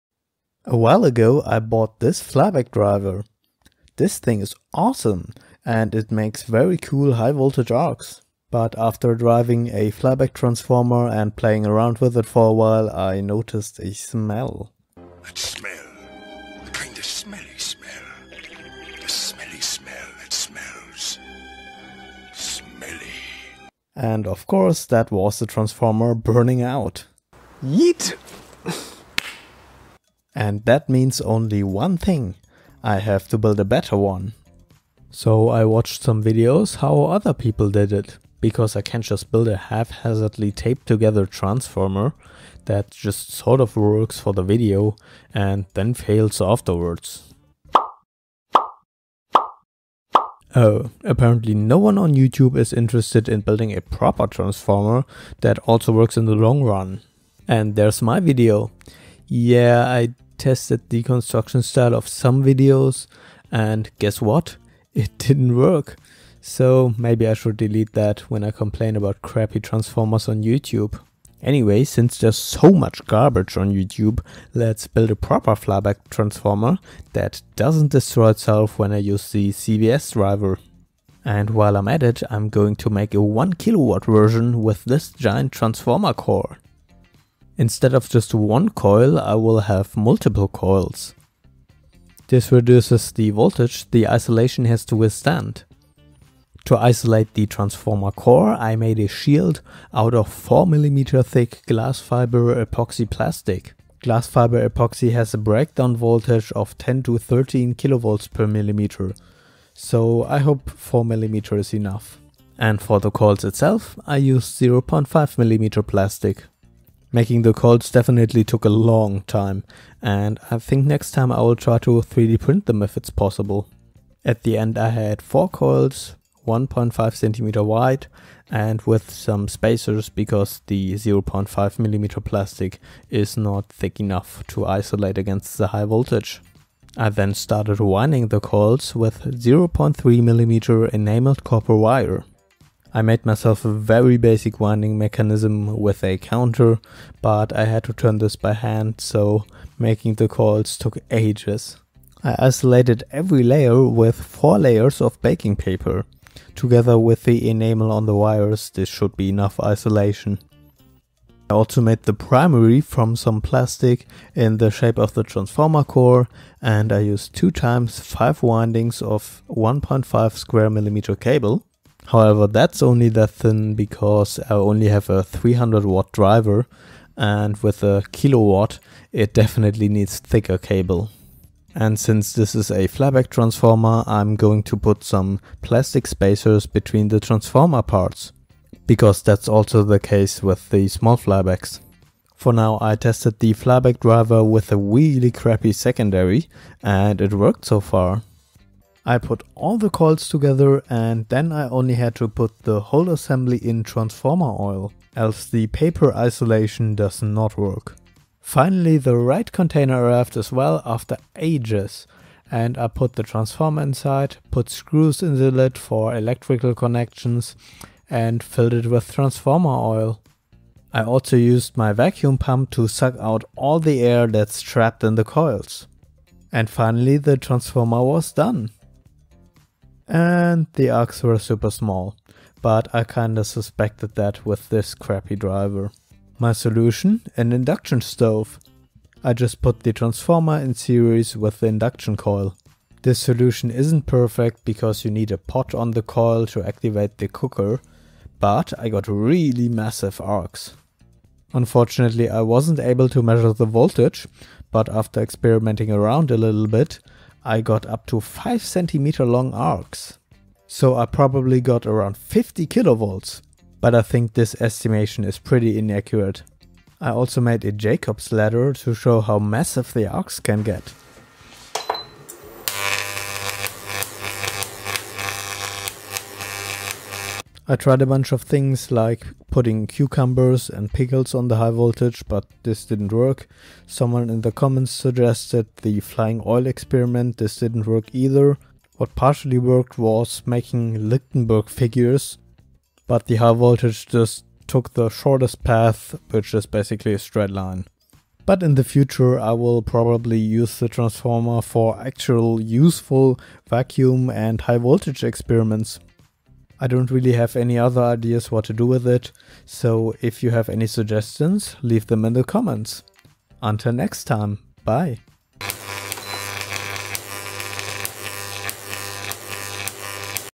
Building a GIANT high voltage flyback transformer
This giant DIY transformer generates up to 50kV with a power of 1kW and it's completely selfmade.